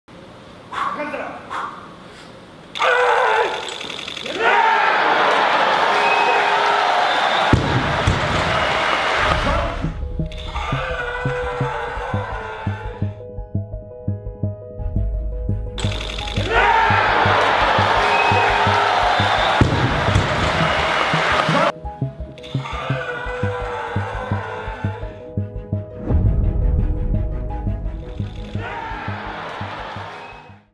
Alexandr Zaichikov 191kg Snatch 2015 World Weightlifting Championships
Alexandr Zaichikov (105kg, Kazakhstan) 191kg Snatch 2015 World Weightlifting Championships